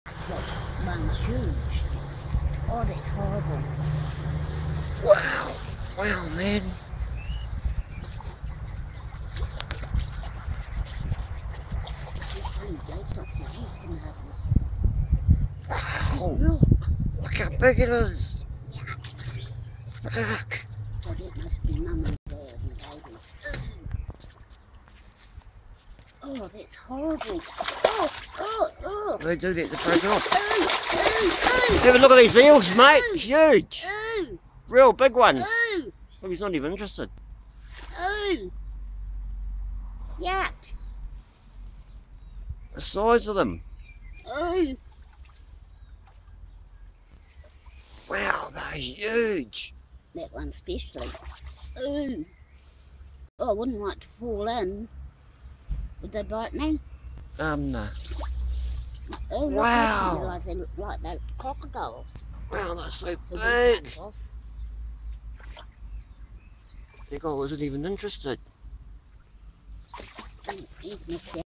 Kiwi River Monsters Attack Sausages
4ft long Kiwi River Monsters (eels) attack a string of sausages i watched floating down our creek. These are apparently not the biggest as our neighbour came over after i filmed this & said the one called 'Titan' was not involved.